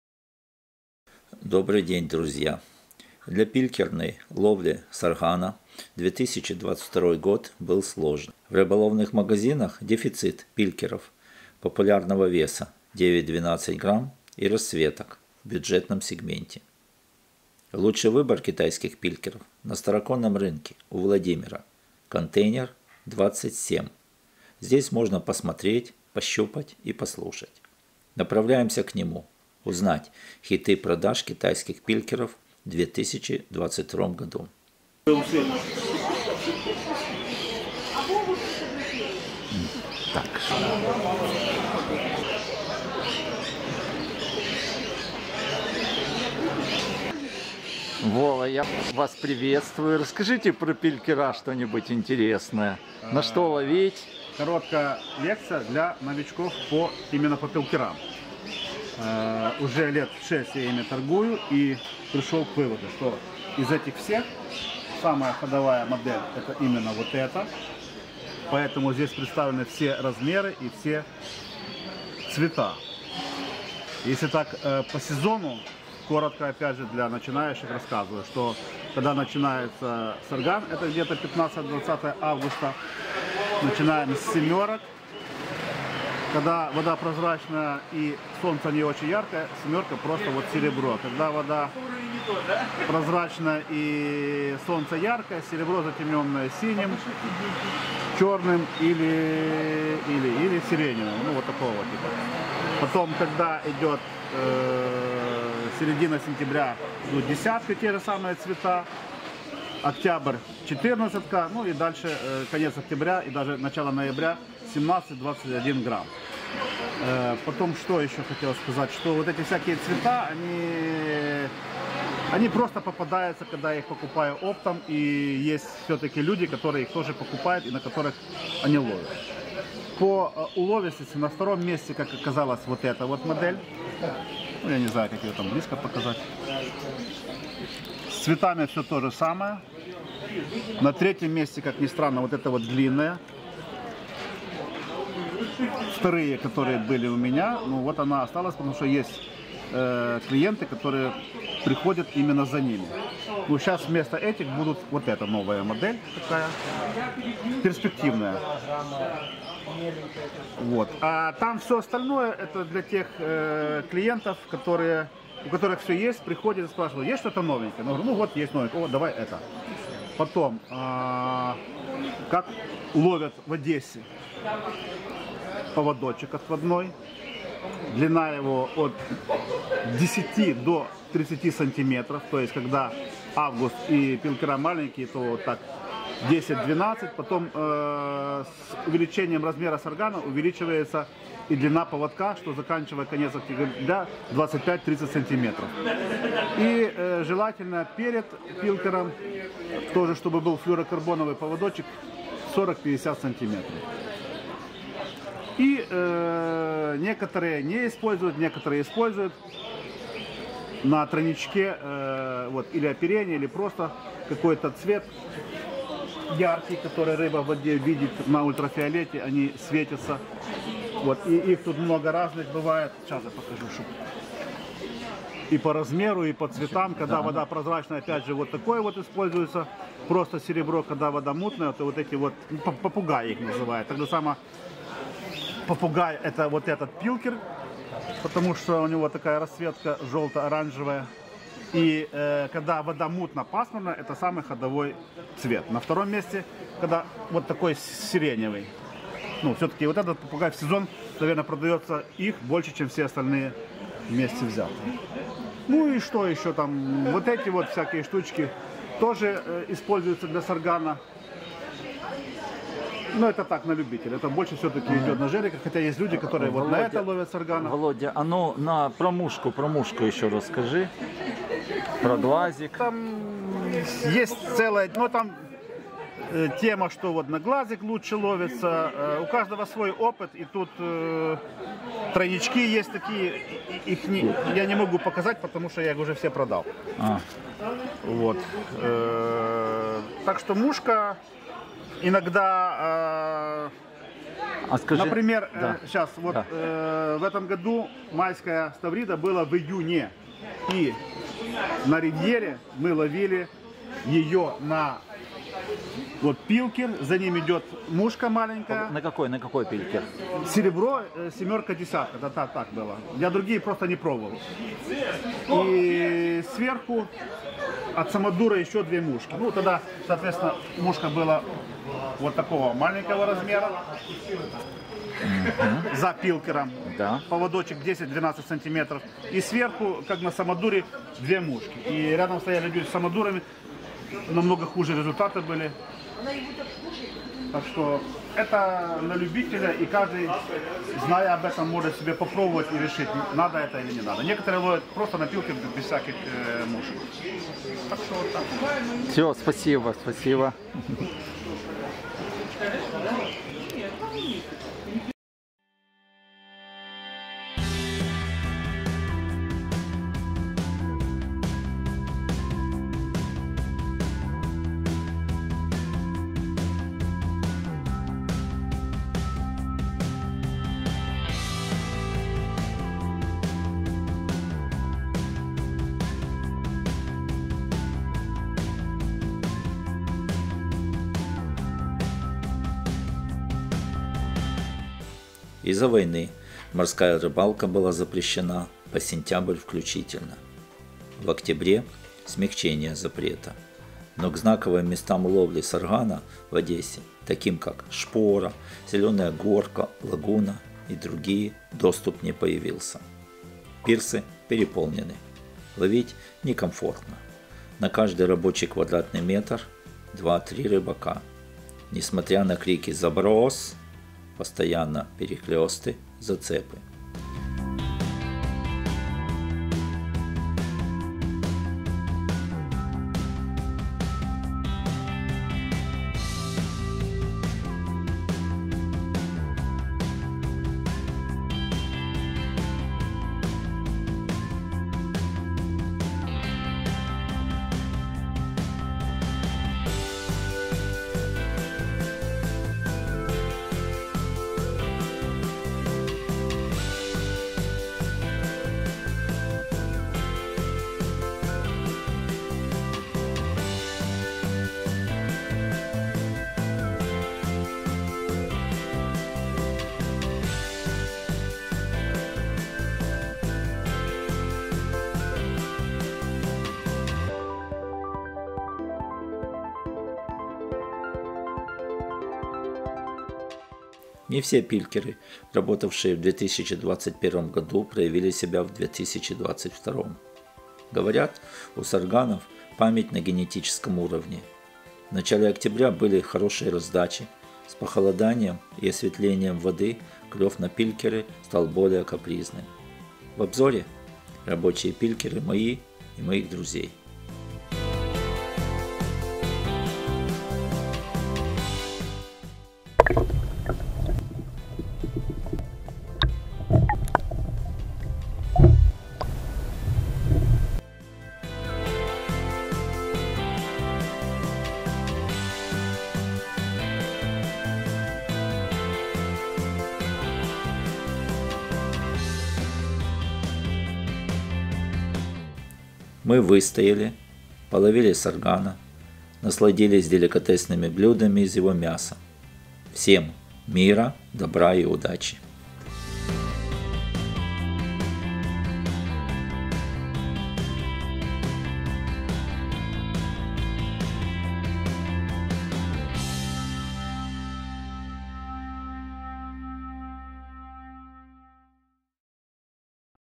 Пилькеры на саргана. Хиты 2022.
Из-за войны морская рыбалка в Одессе была запрещена по сентябрь включительно. В октябре смягчение запрета. Пирсы переполнены. Не все пилькеры 2021 года проявили себя в 2022. Говорят у сарганов память на генетическом уровне. В начале октября были хорошие раздачи. С похолоданием и осветлением воды клев на пилькеры стал более капризным.
 Лучший выбор бюджетных пилькеров на Староконном рынке у Владимира (контейнер 27). Направляемся к нему - узнать хиты продаж китайских пилькеров в 2022 году.